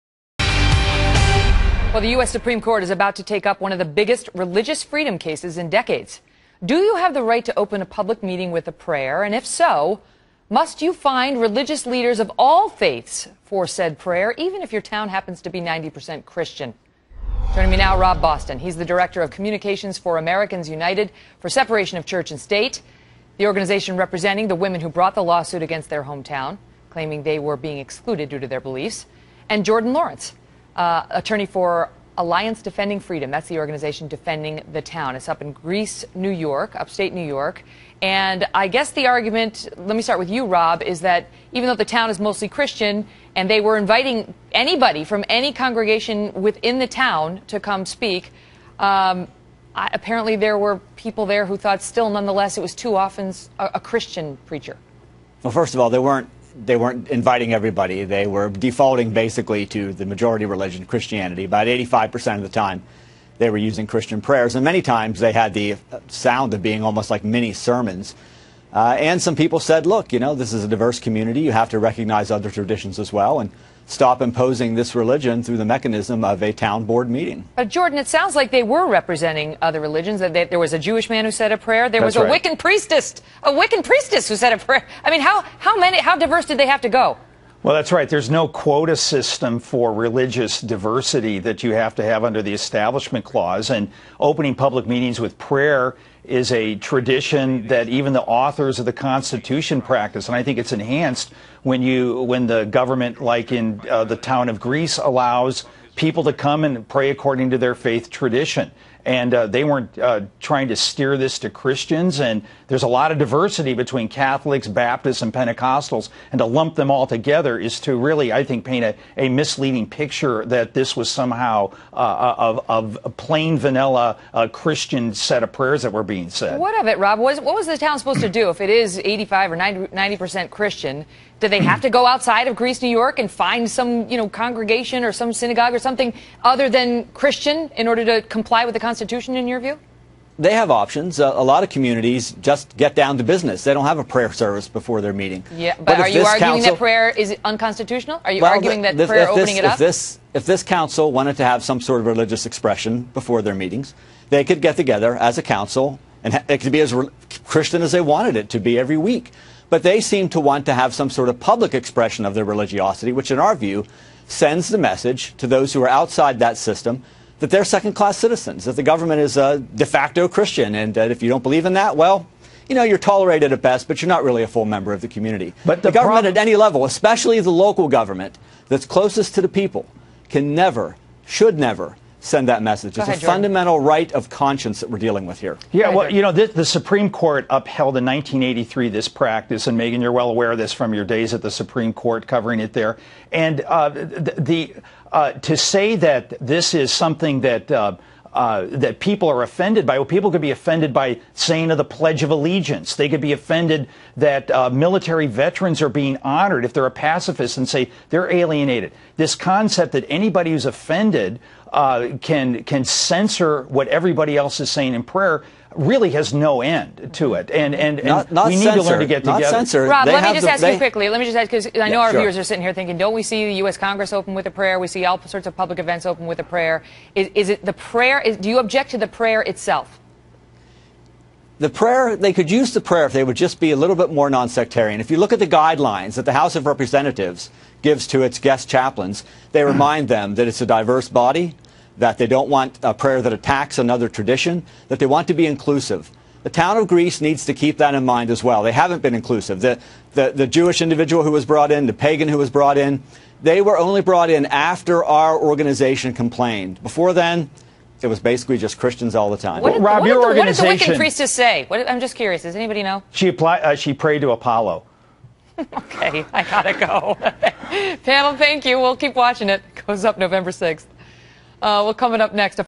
U.S. Supreme Court : Vote whether prayers should be permitted before public meetings (Oct 11, 2013)
News Articles:

Supreme Court: Campaign finance and public prayer top new term agenda

U.S. to argue in N.Y. town's prayer at meetings case